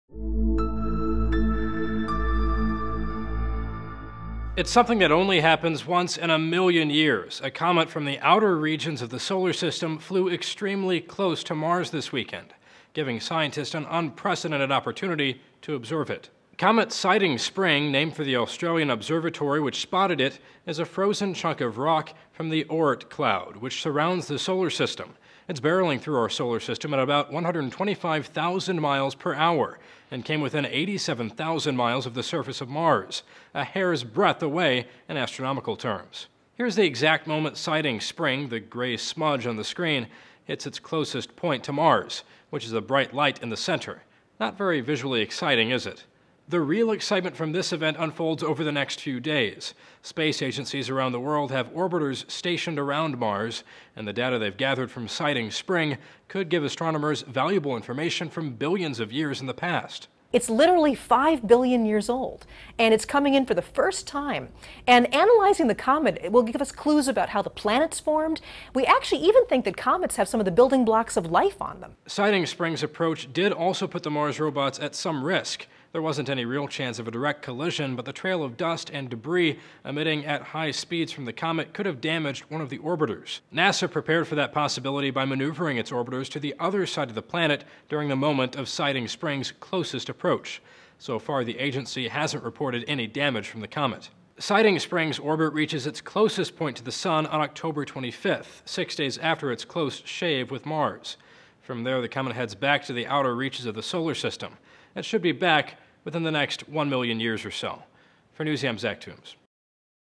Comet Siding Spring Grazes Mars' Atmosphere
A comet from the farthest reaches of the solar system passed extremely close to Mars this weekend, giving astronomers a rare opportunity to study it.